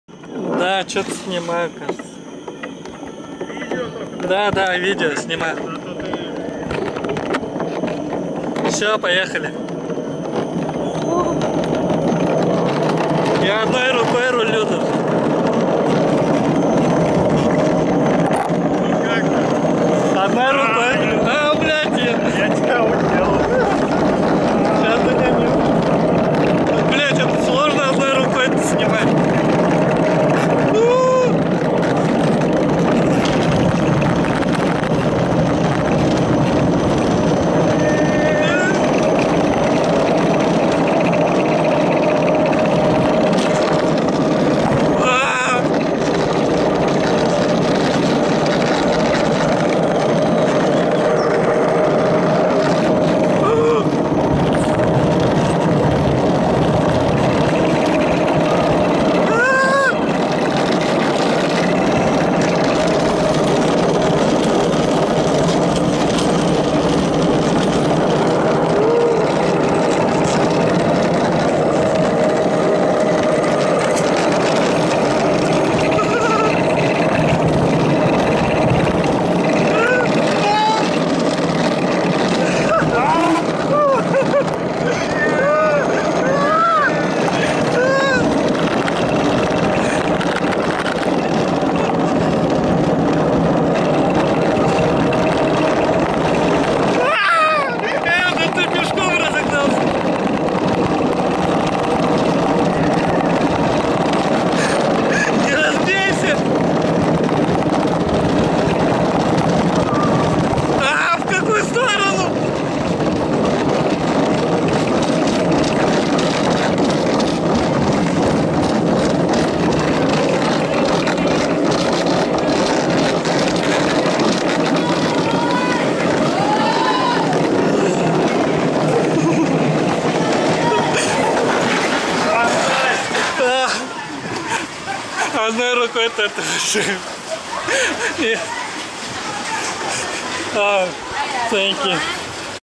Luge Ride Sentosa / Гонка на Санях, Сентоза
Perfect competition downhill on a luge.
Special thanks to Denis for the record.
Прекрасное соревнование по скоростному спуску на санях.
Особая благодарность Денису за предоставленную запись.